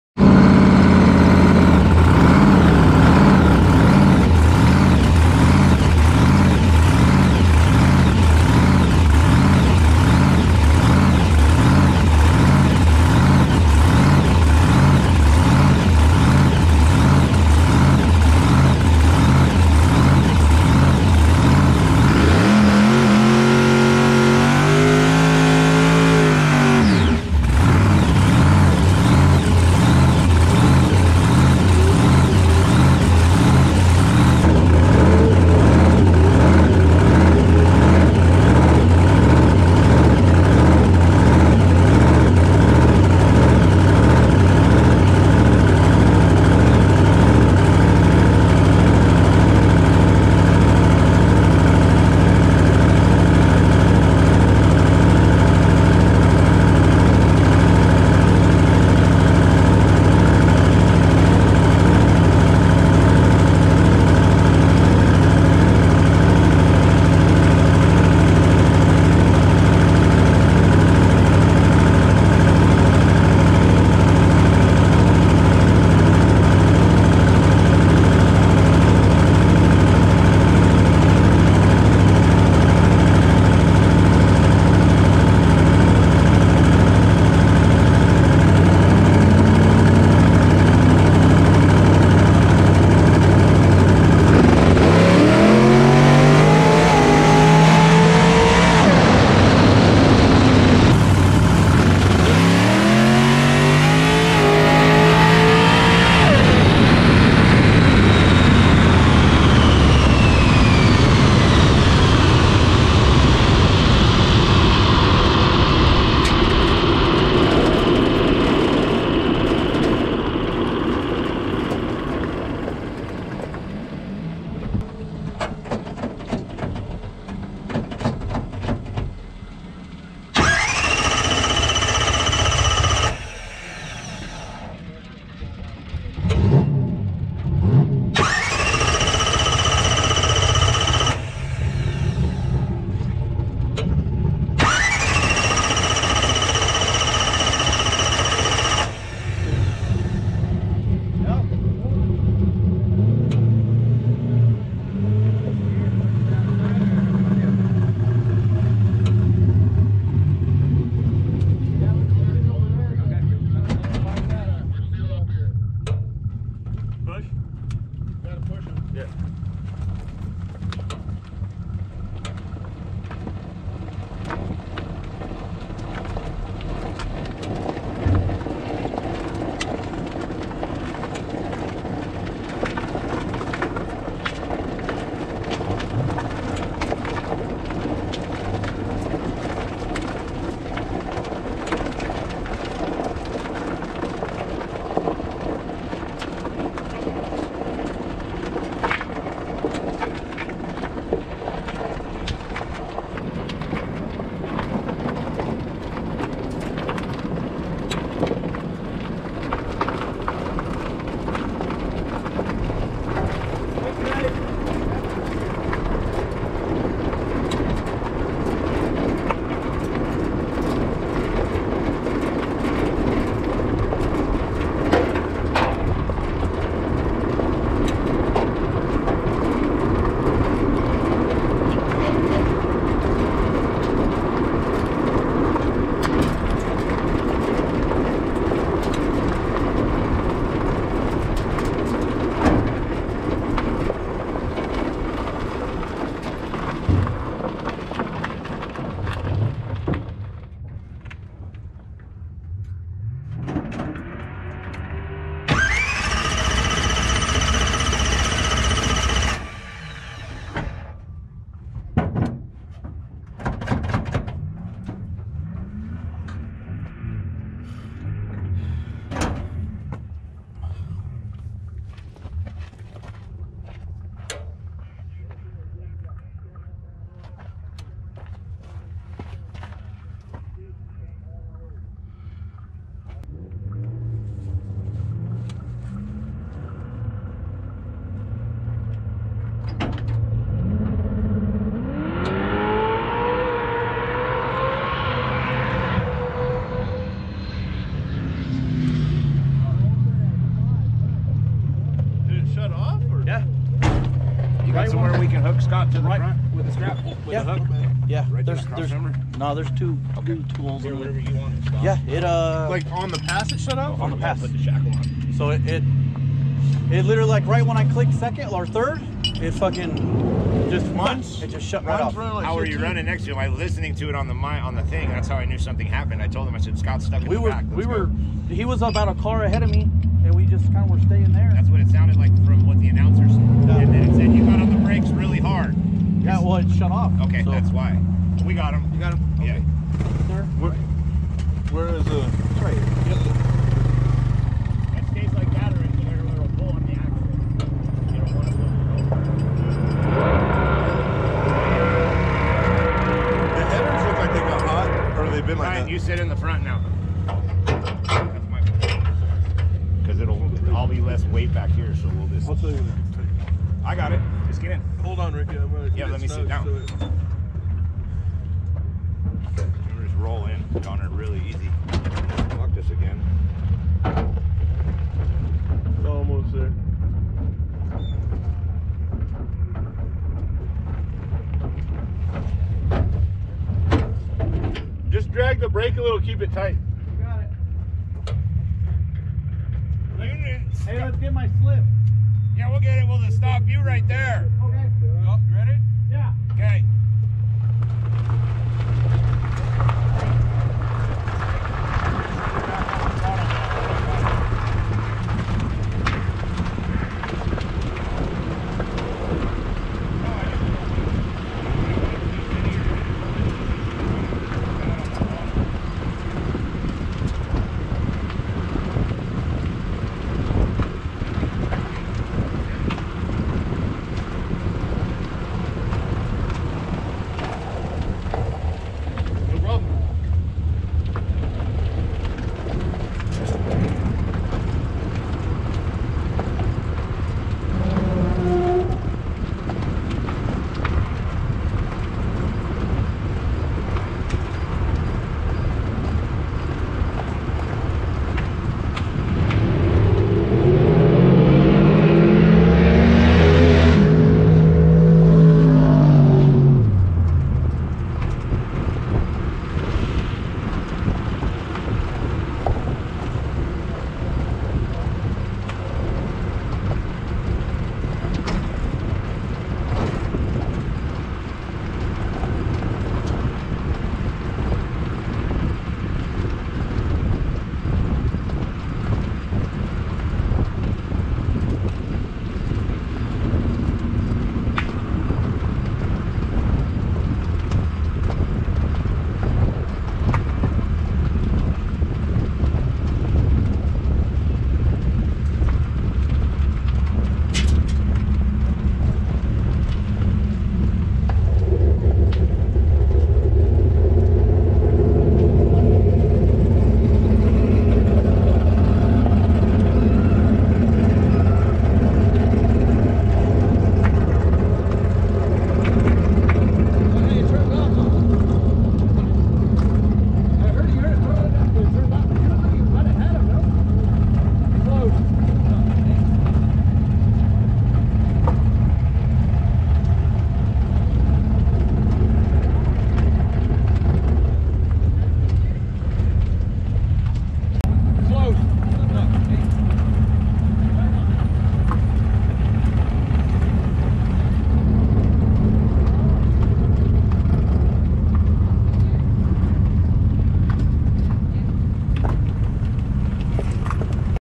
Racing King Donnie, breaking, saved by the 3 stooges
Good race with king Donnie till car shut off. After checking I lost the crank sensor. Glad my buddies came to the rescue